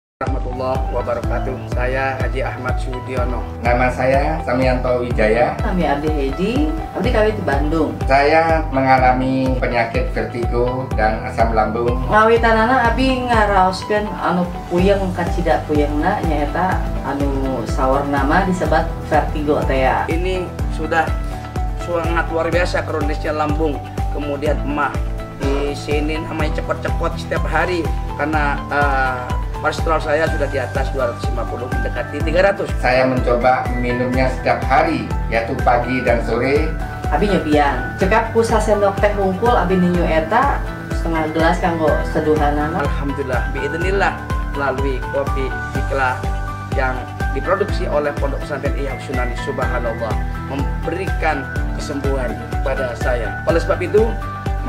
minum kopi bikla bjsa menyembuhkan segala macam keluhan pengeluaran jadi penghasilan
Bpk haji Achmad Sudiyono *(Bupati LIRA)* pd pameran UMKM one Pesatren one product 21 Pebruari 2021 di Ponpes Nurul Quran Rowotamtu Rambipuji Jember tempat mondok Pertama kali KH.Imam Bukhori sebelum Ke Salafiyah Syafi iyah Situbondo.. Luar biasa KOPI BIKLA💯☕💸💵💰👏
Wa .085288777990